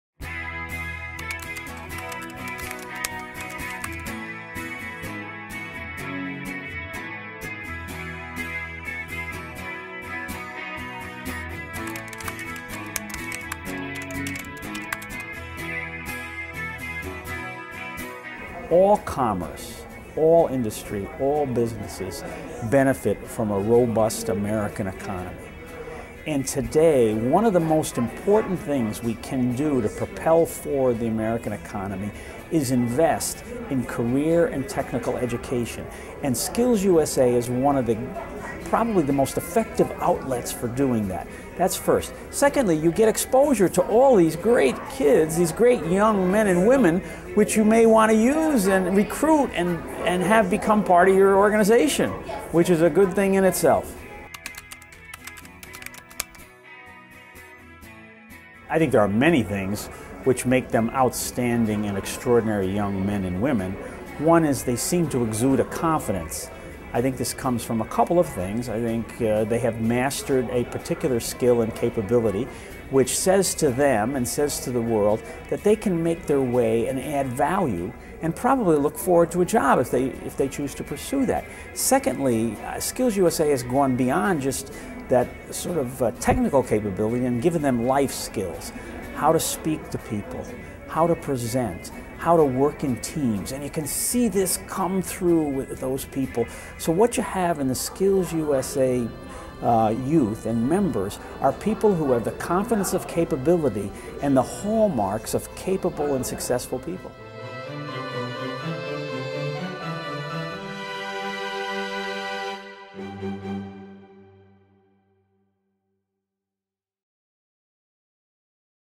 Nick Pinchuk, chairman and CEO of Snap-on Inc., on the importance of SkillsUSA
Nick Pinchuk, chairman and CEO of Snap-on, was named SkillsUSA's Champion of the Year in 2012. Here, Pinchuk offers some thoughts about why business and industry should support SkillsUSA and what sets SkillsUSA students apart from their peers.